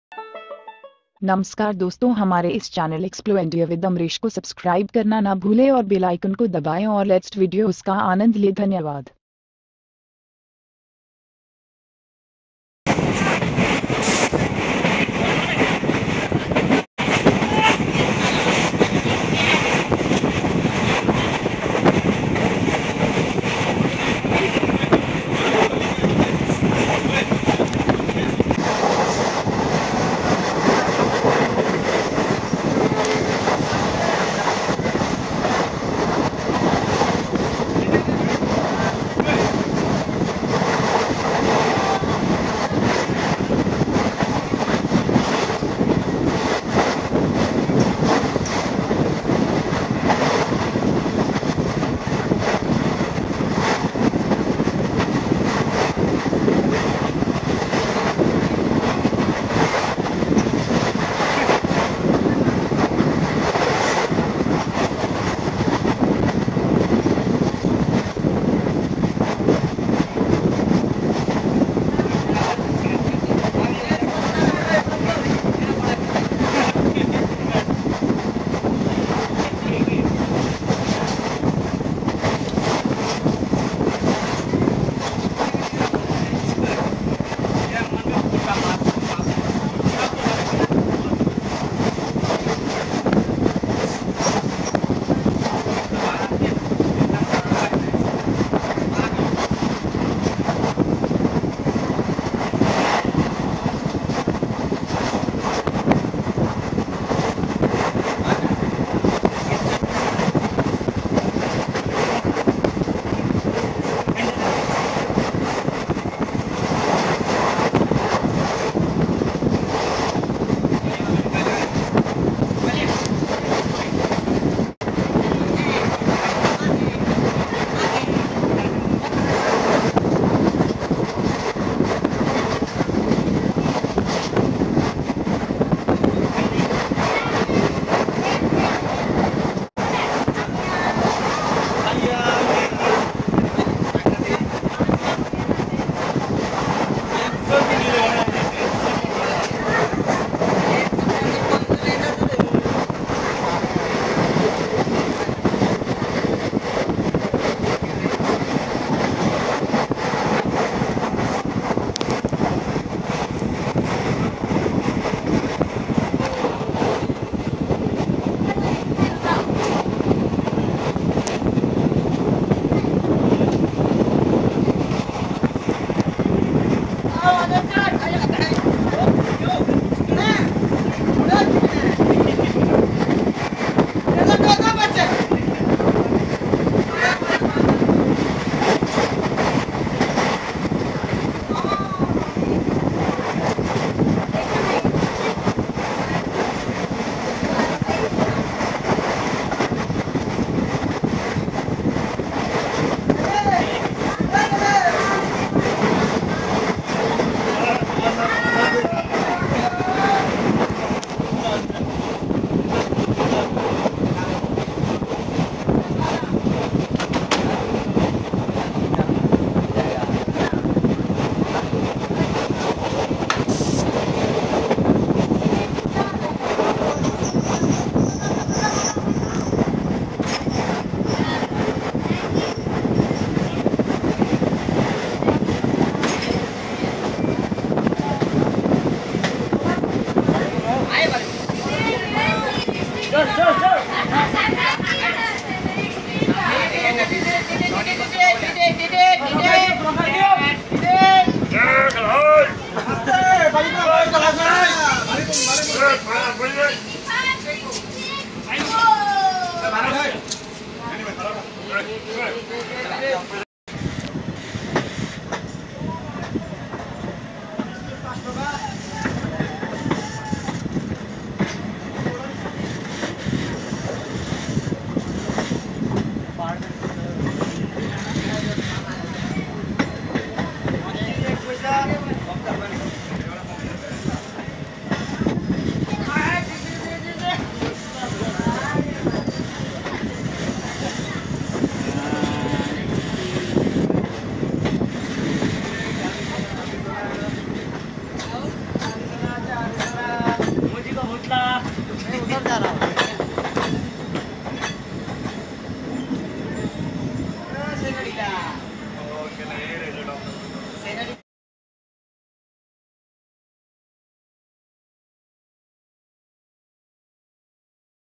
Rangapara North to Bhalukpong by train || 55719 Dekargaon-Bhalukpong Passenger
A beautiful journey from Rangapara north junction ( Assam ) to Bhalukpong ( Arunachal Pradesh ) and amazing view of a forest, river and hills during the journey.